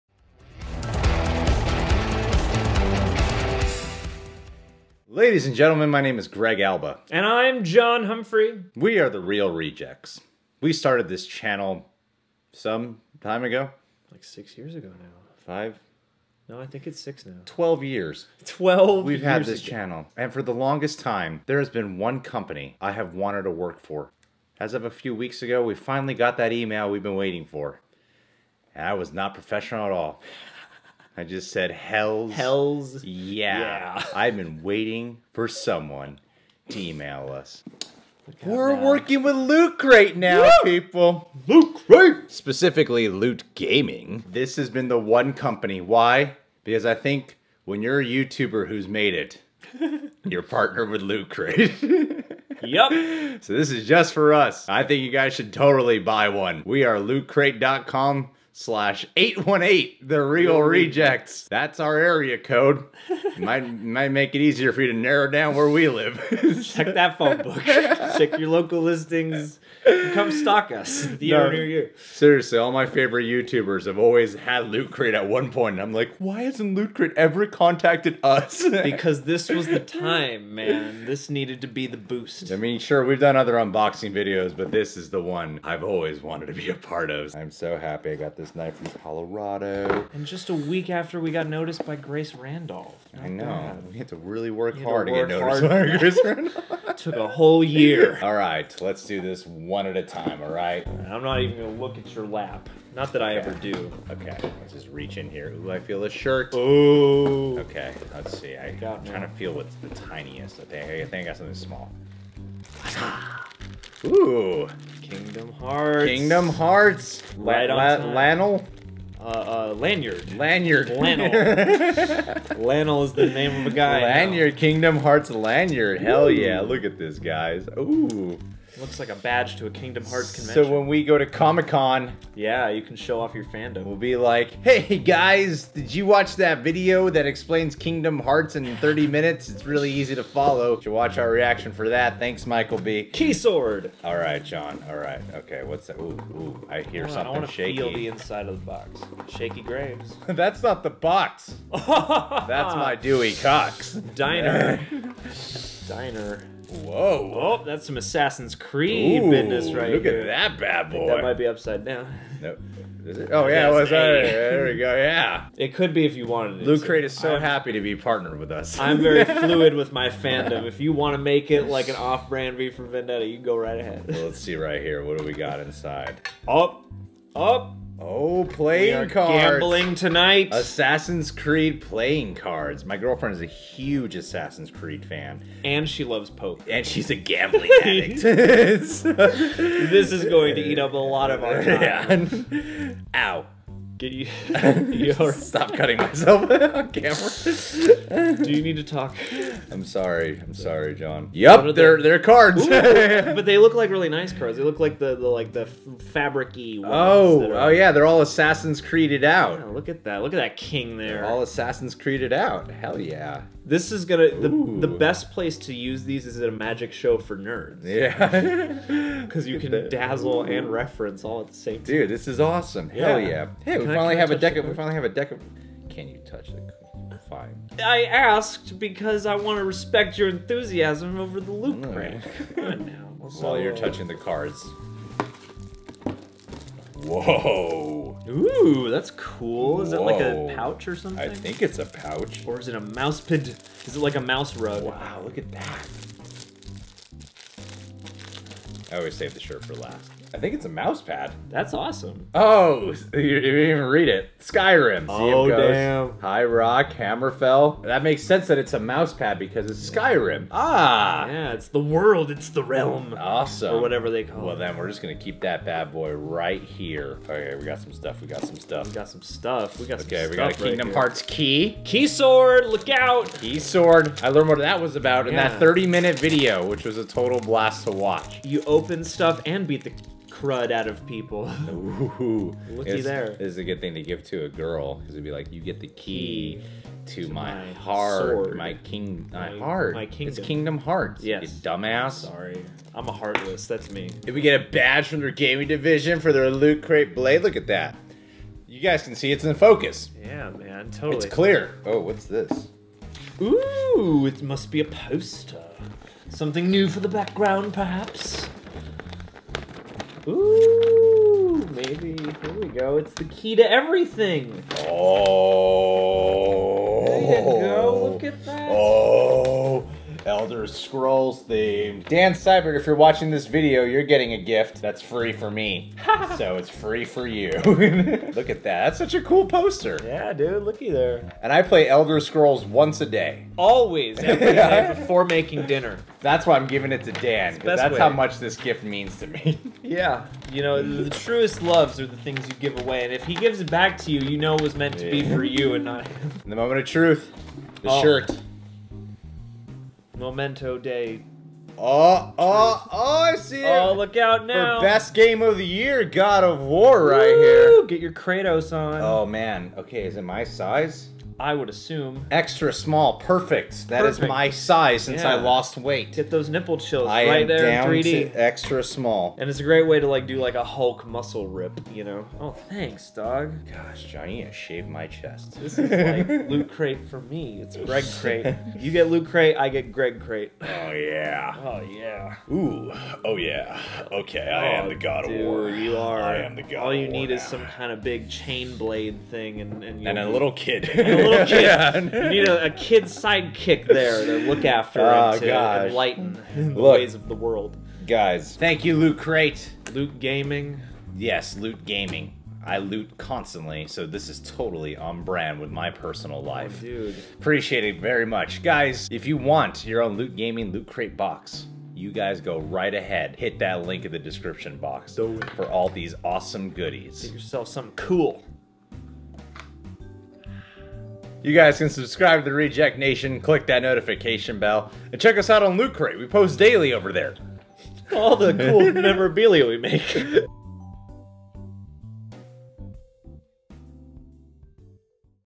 UNBOXING - LOOT GAMING 2018 "Blade" Crate!!!
So here's a li'l holiday treat that presented itself to us just recently... we're finally workin' with Loot Crate & Loot Gaming! hit up to sign up & use offer code "818thereelrejects" to save 10% on your subscription!!!